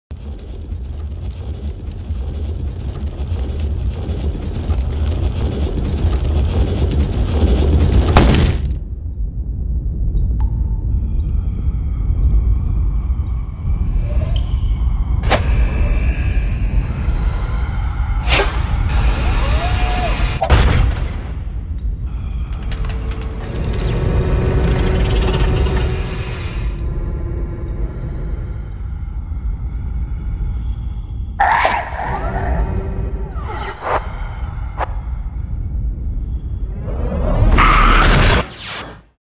2002 Bohrok Character Spot: Lehvak
Character spot for Lehvak, from the Bohrok promo CDs